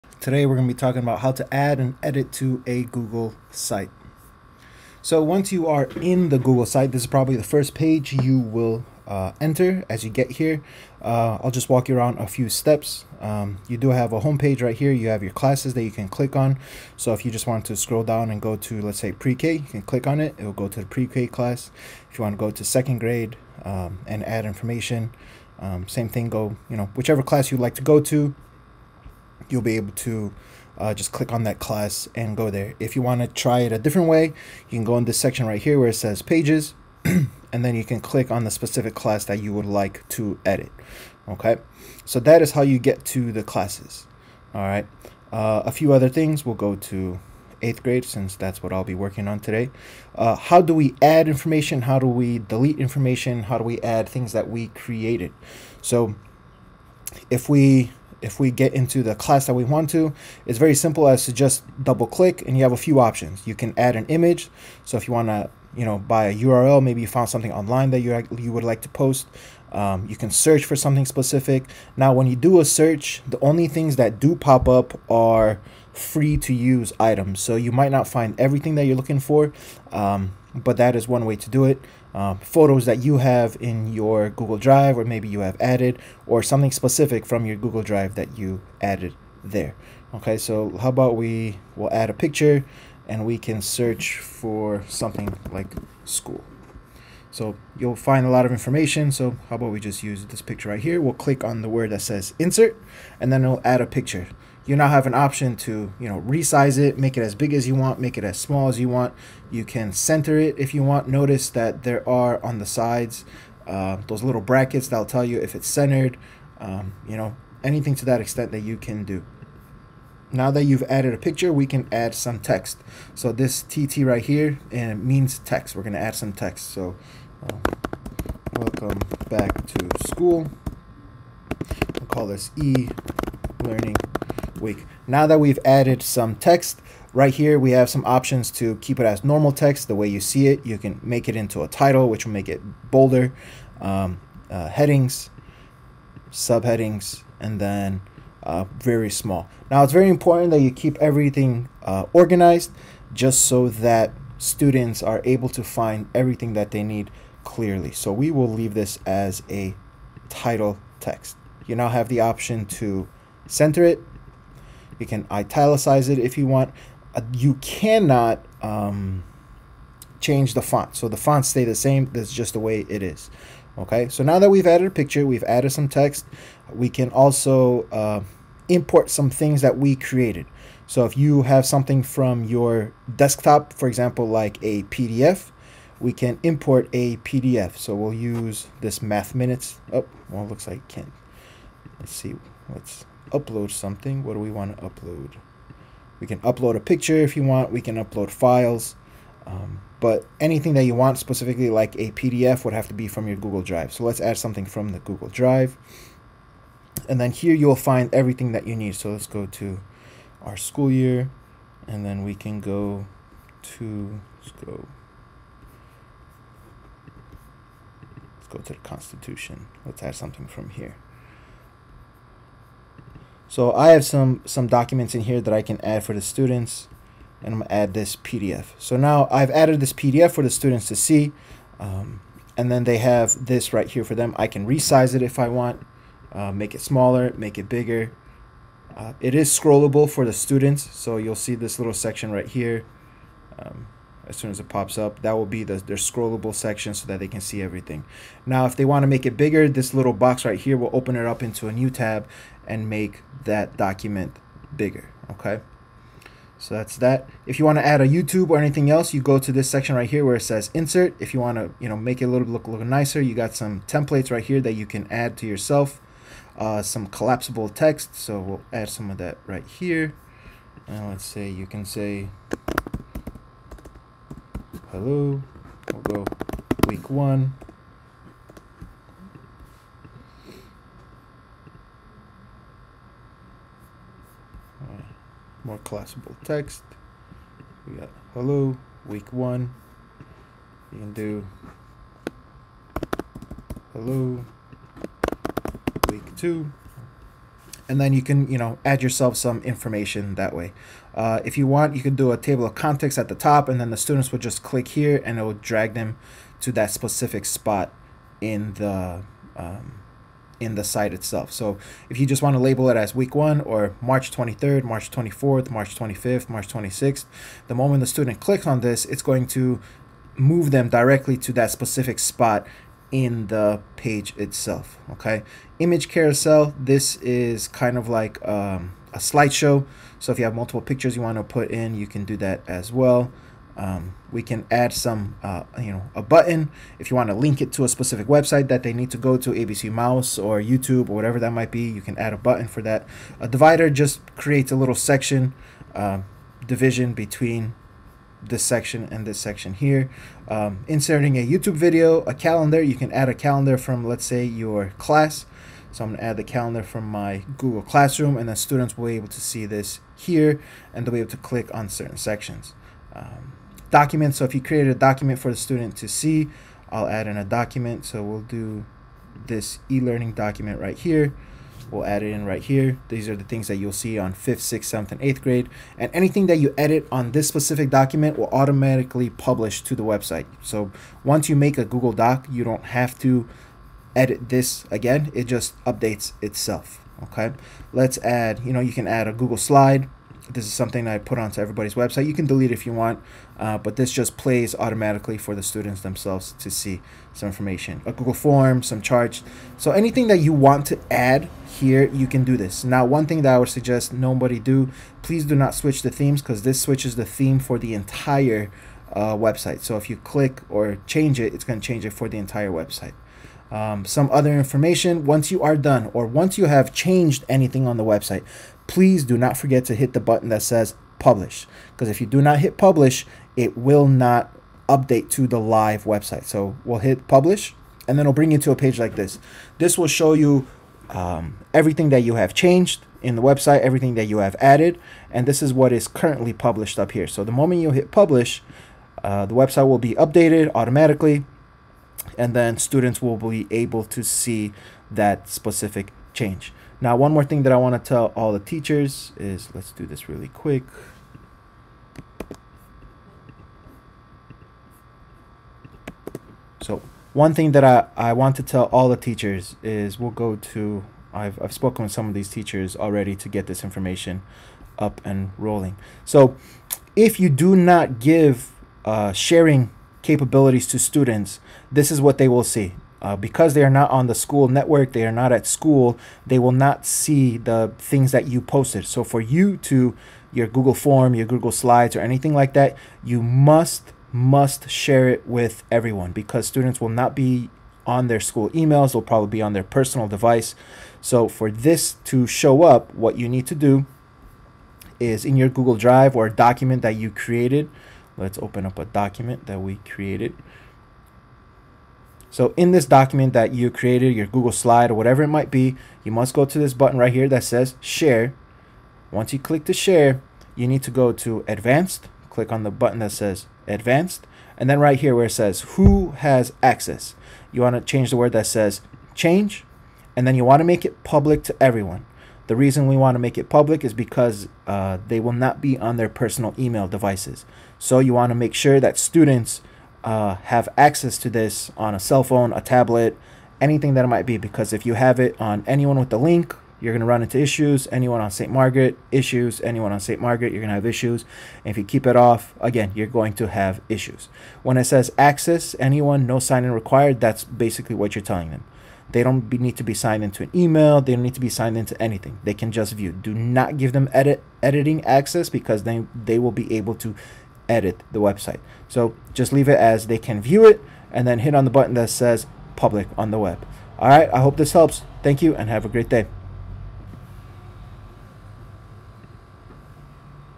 How to Add and Edit a Google Site
Today we are talking about how to add and edit a google site. You will see all the functions of a google site. How to get to your specific page. How to add text, change text move text and resize text. You will also learn how to add a picture, resize add google drive and pdfs to the website. You will also learn how to publish to the web.